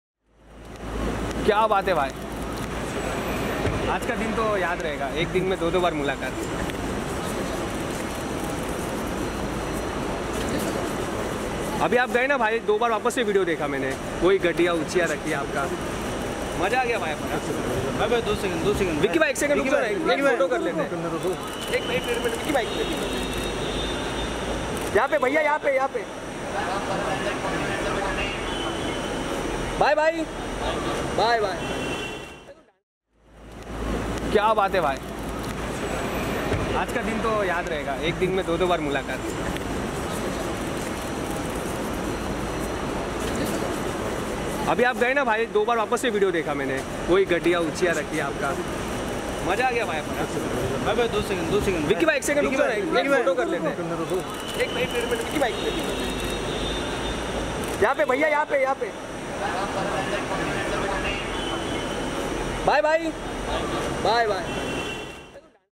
Vicky Kaushal Visiting Ahmedabad To Watch IPL Final Match
Vicky Kaushal was spotted going to Ahmedabad to watch IPL Final.